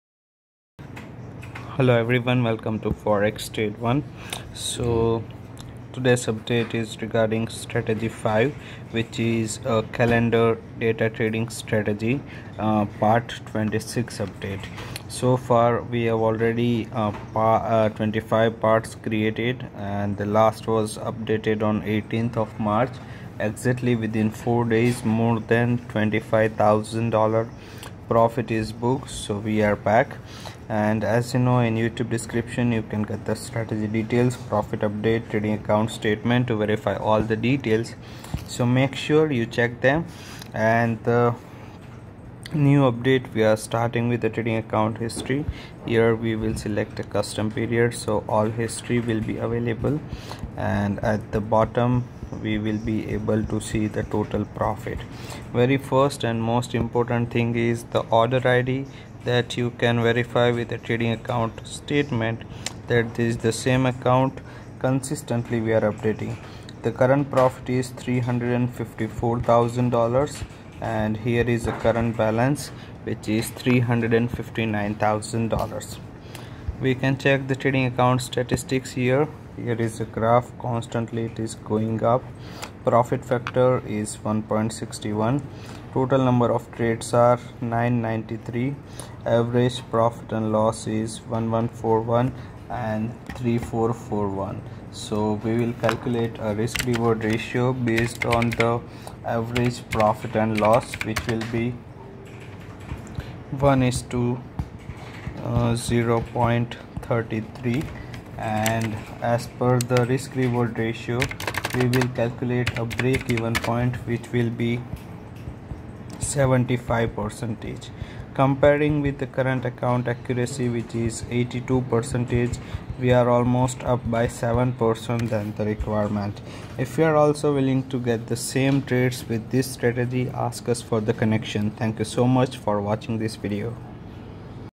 STRATEGY 5 Calendar Data Trading Strategy - 219% ROI Increased in 4 Day with Forex Trading PART 26
Chapter
00:00 Introduction
00:10 Verify Previous Update
00:40 Trading Account History
01:15 Trading Account Statement

Mostly traded in following pairs:
XAUUSD
GBPUSD
USDCAD
EURUSD
USDJPY
NZDUSD
USDCHF
AUDUSD